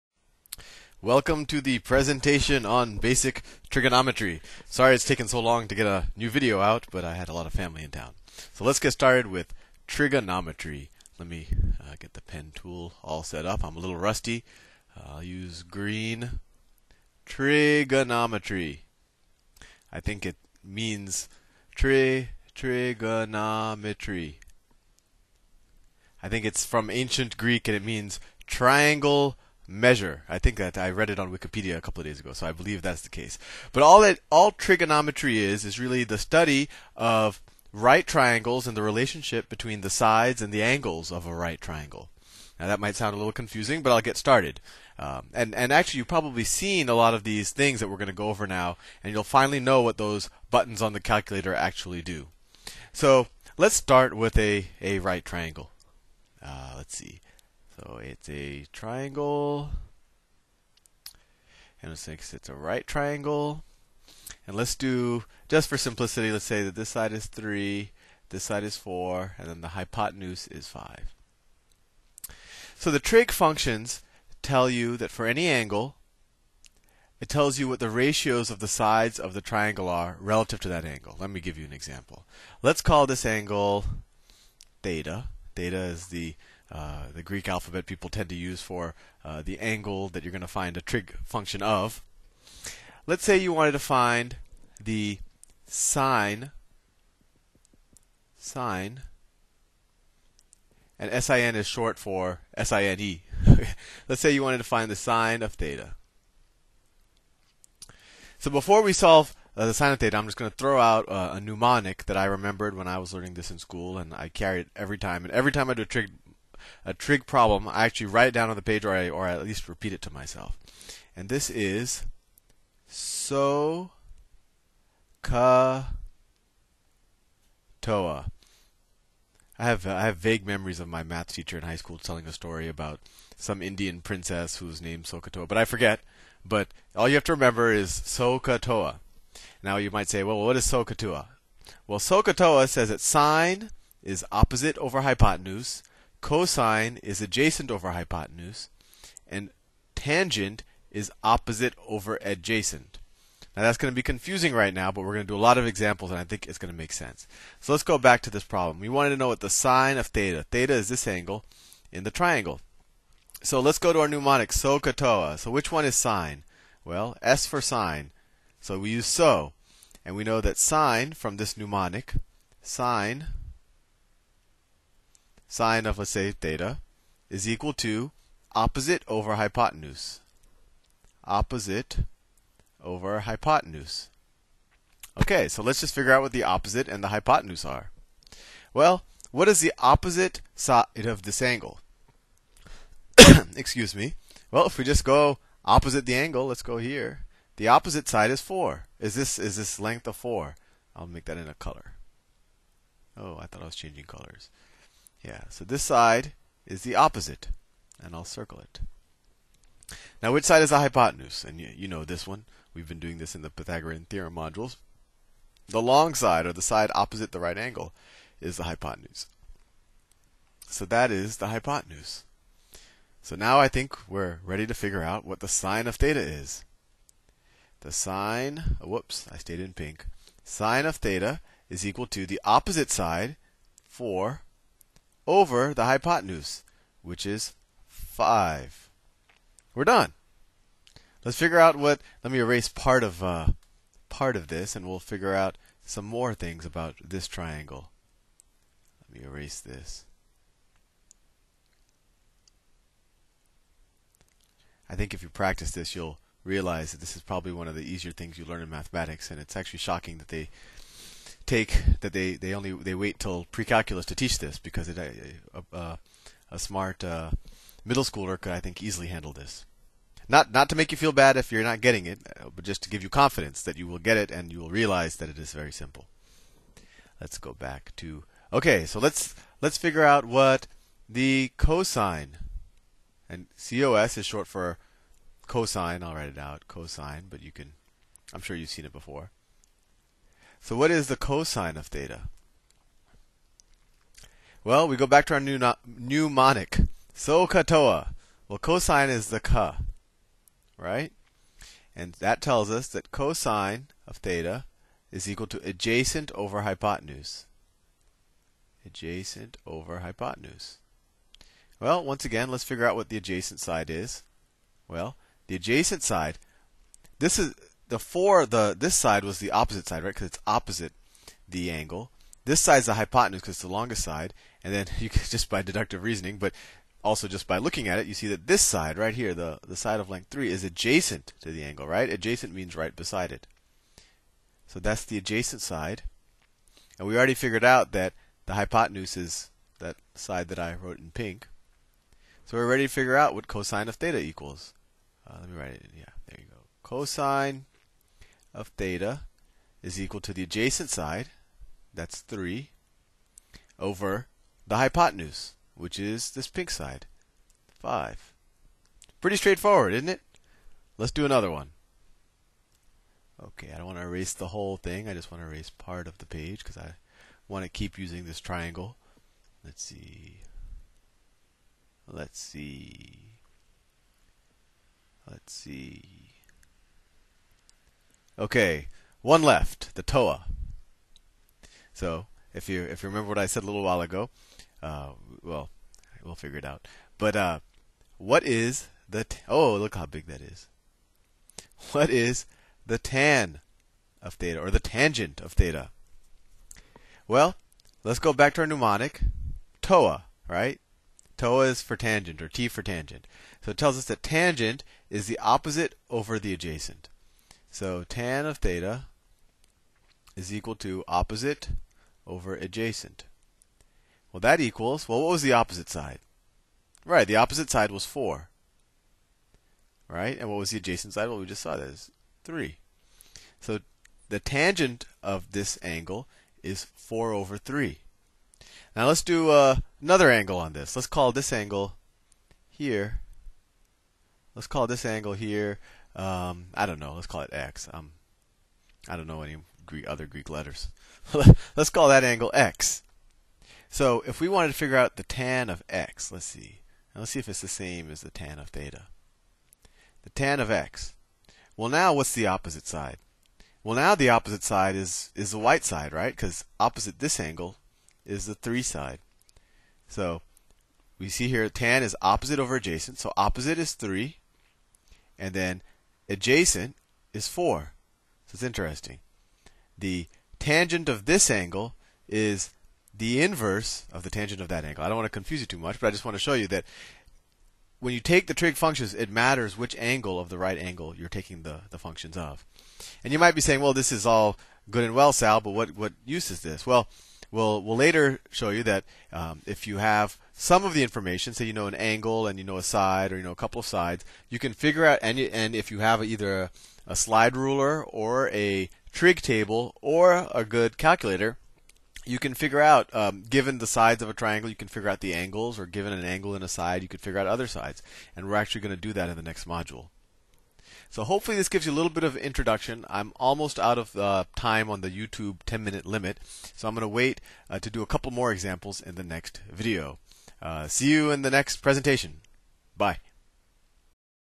Basic Trigonometry
An introduction to trigonometric functions: sine, cosine, and tangent.